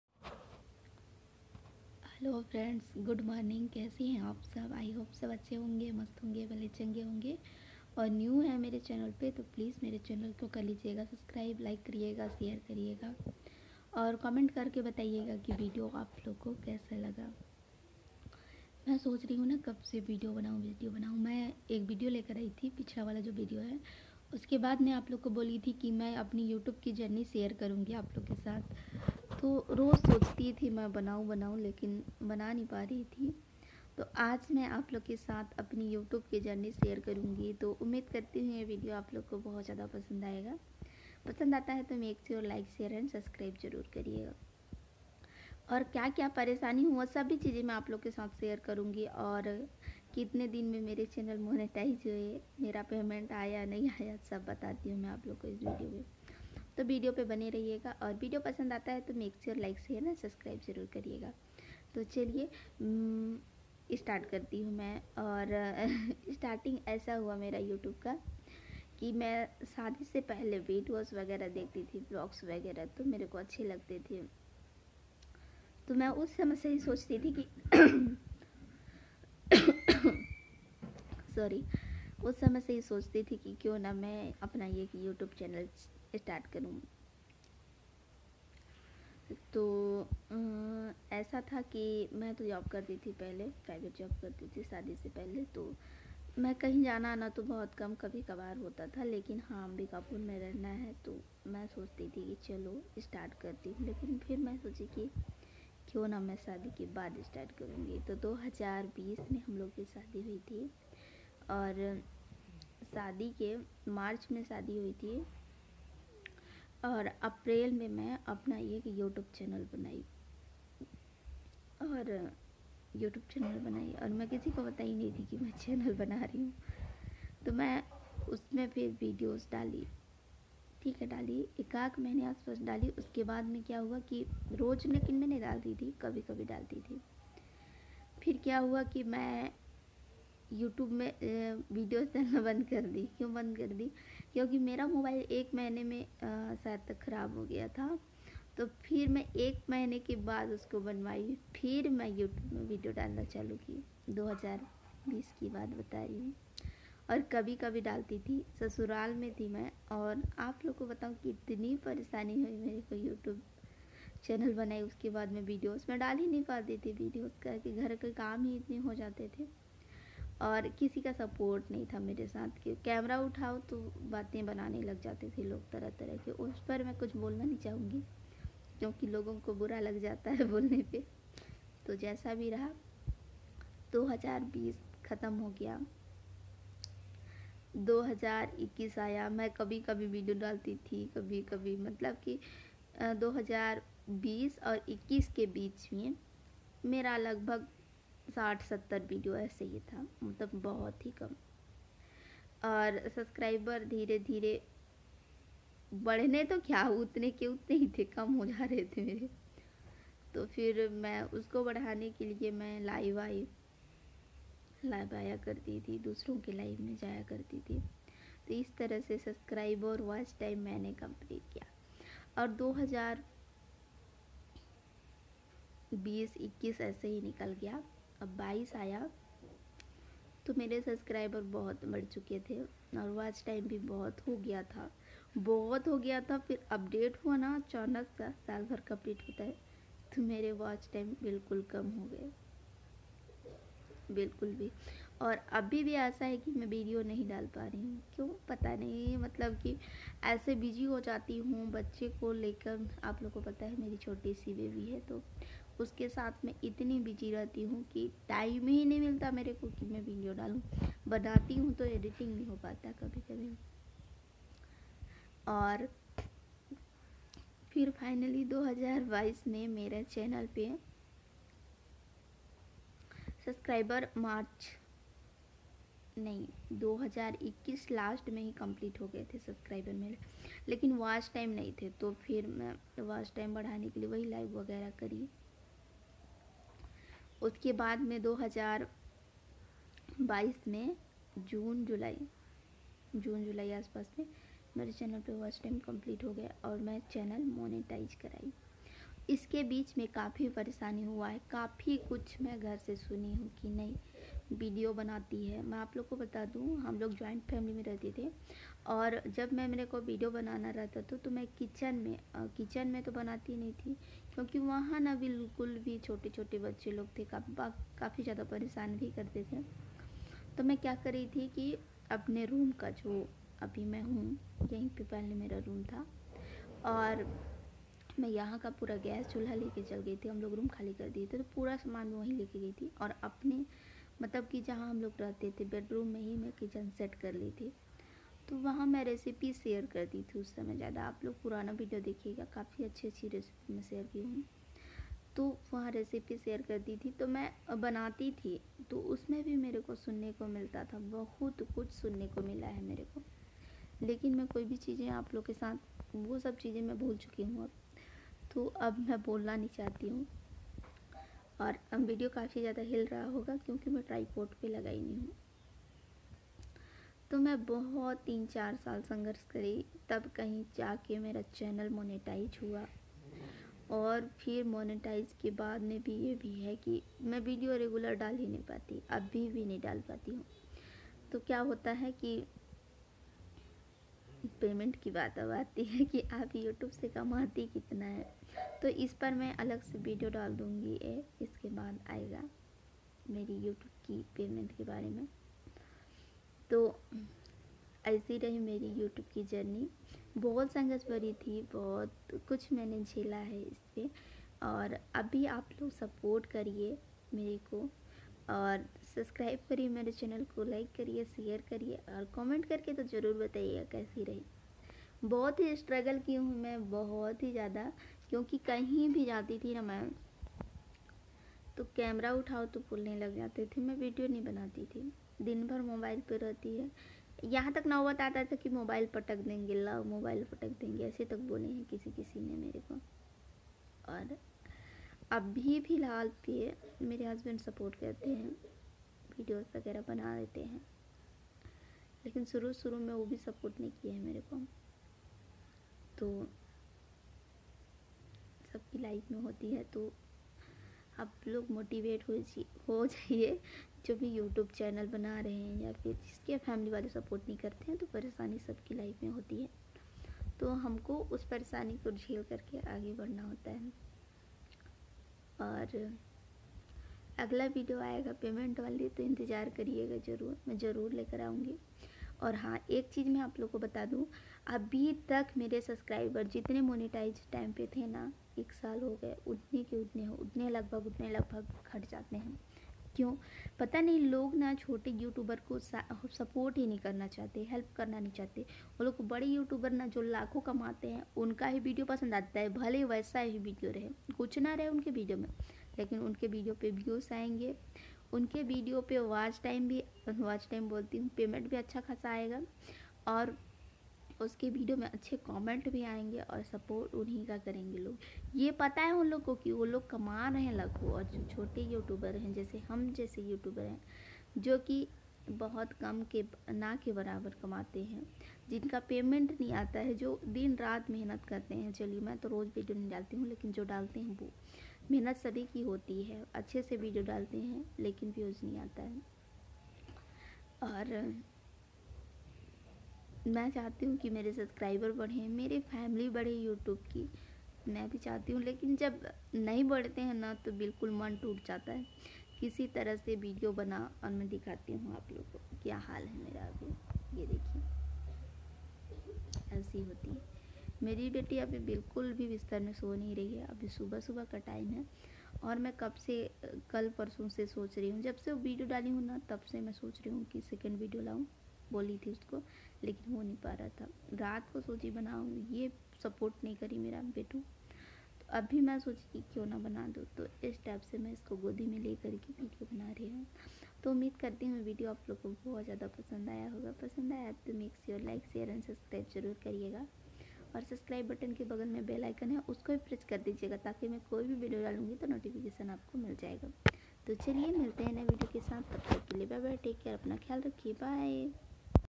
मेरी यूट्यूब की यात्रा 😭😭 YouTube ki journey 🙏🙏@saritavlogs5358
hello friends 🙏🙏🙏🙏🙏
aaj main aap log ke sath apni YouTube ki journey share kar rahe hain aap log ko pasand aaega jaisa bhi lagta hai comment karke jarur bataiyega aur video pasand aata hai to like shar and subscribe jarur kariyega 🙏🙏🙏🙏🙏🙏🙏